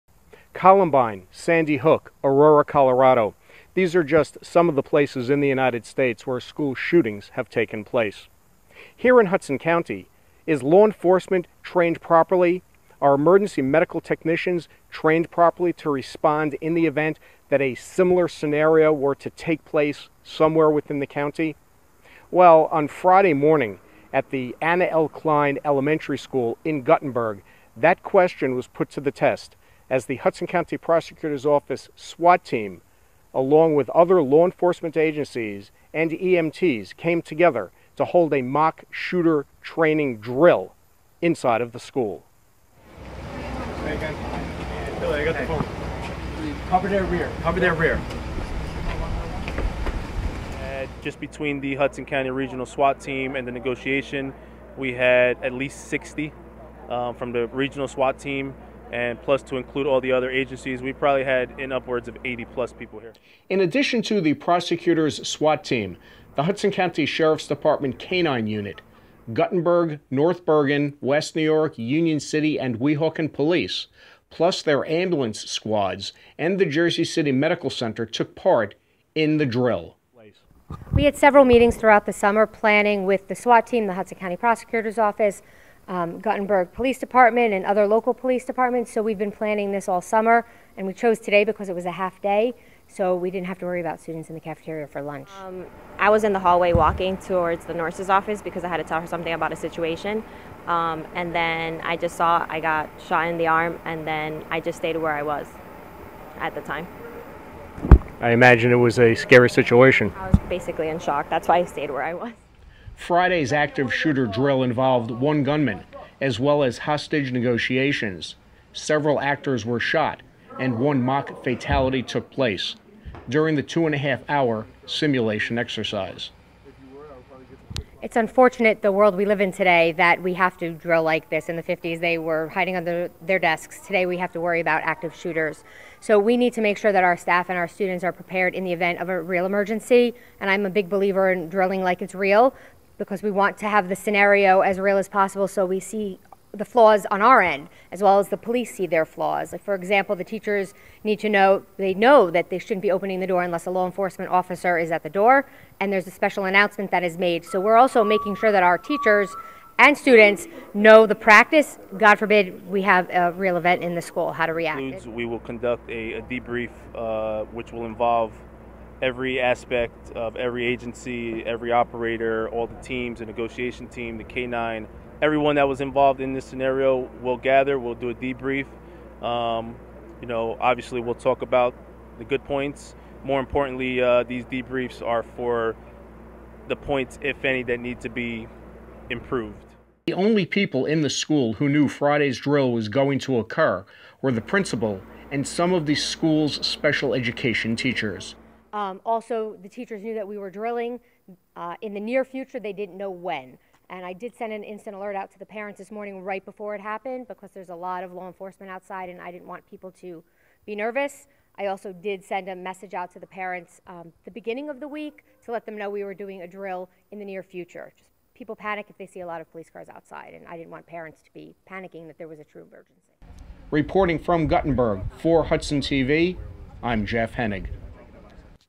S.W.A.T. TEAM HOLDS ACTIVE SHOOTER DRILL IN GUTTENBERG
Hudson County law enforcement personnel and local emergency medical technicians participated in a mock, active shooter drill at a local elementary school on Friday morning.  Hudson TV reporter Jeff Henig has the story.